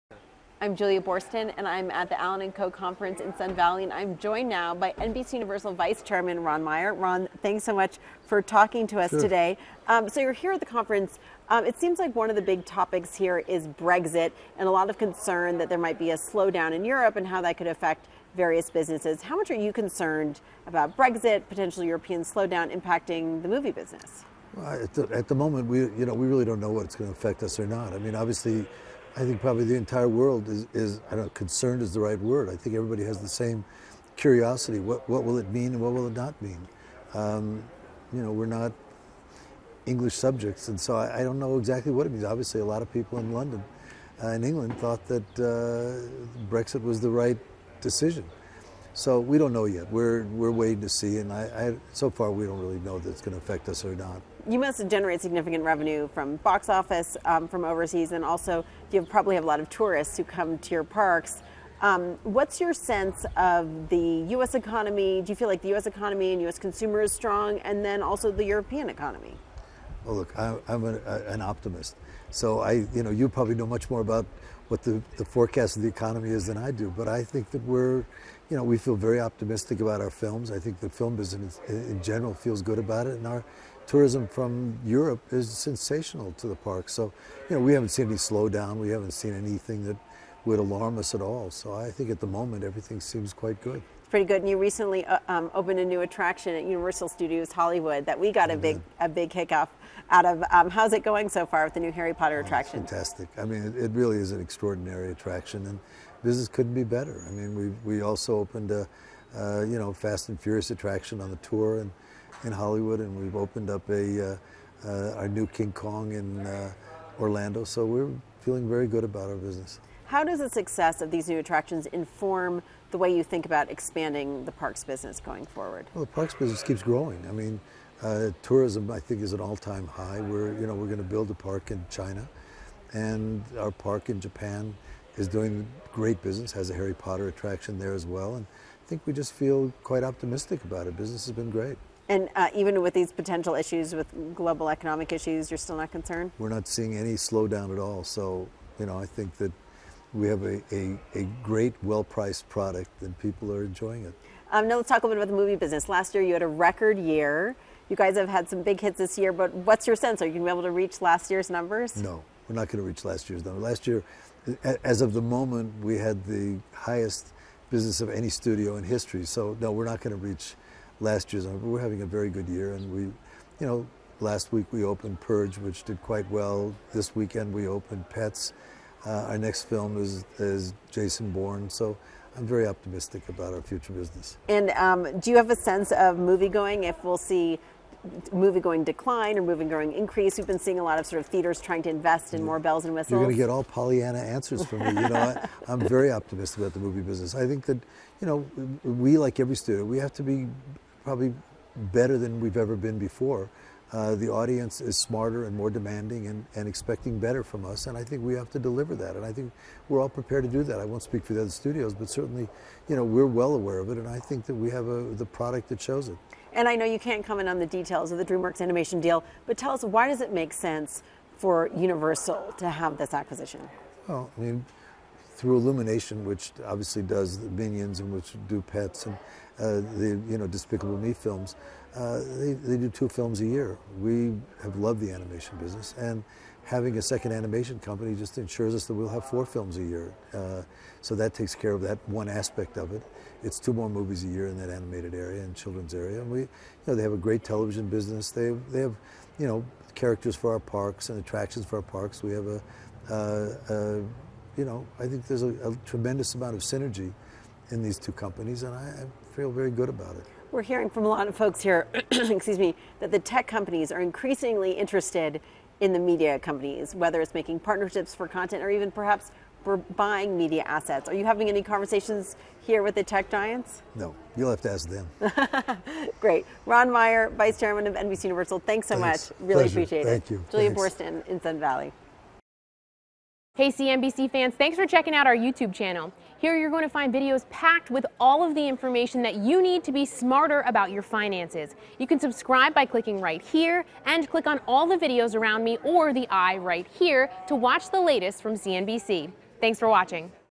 NBCUniversal's Ron Meyer Talks Future Of Movies, Amusement Parks | CNBC
At the annual Sun Valley conference, NBCUniversal Vice Chairman Ron Meyer talks about the latest industry trends, and explains why his company bought DreamWorks Animation.